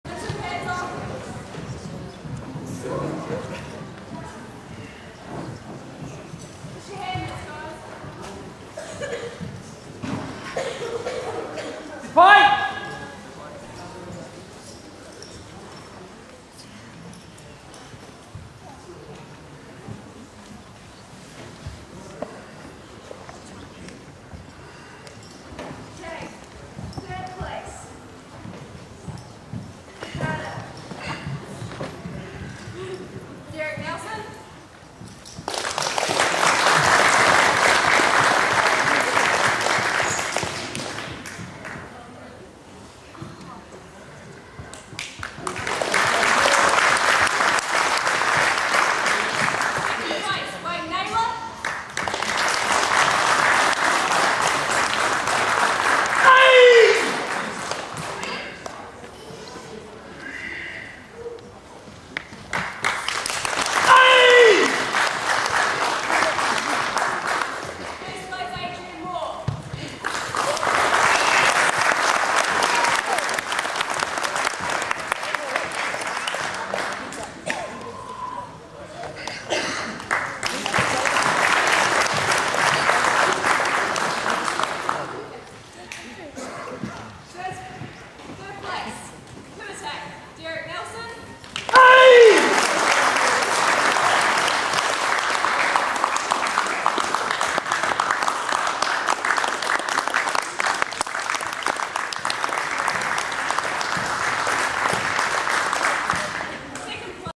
gkr wgtn r2 18 08 2007 ring2 open male kata 13a sepai
GKR Karate
Wellington, New Zealand
Round 2
Tawa College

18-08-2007

Ring2 Open Male (inc Instructor Male and 4th kyu+) Kata and Kumite